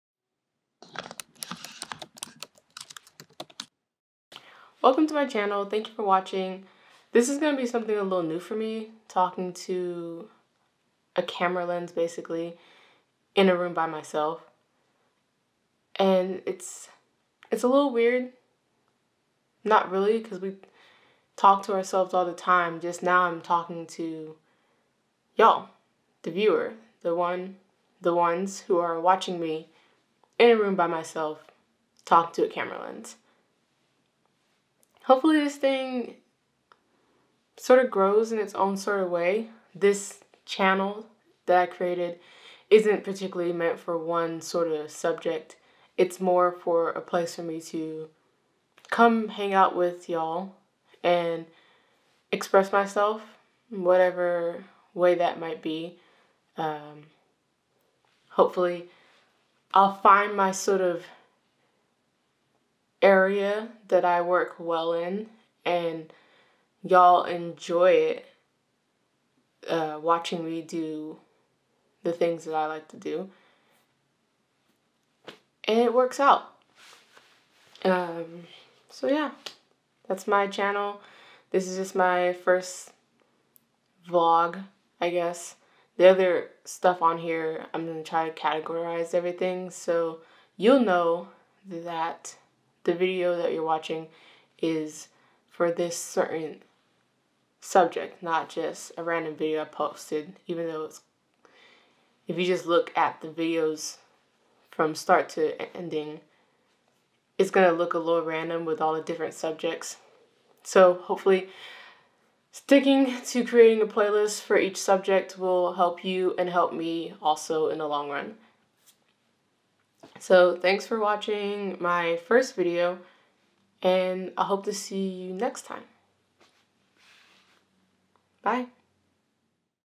Welcome To My Channel | Lee Adams
This is my welcome video to those of you watching. Thank you for the view. I plan on creating different things and doing vlogs, talking about things that interest me that I want to share.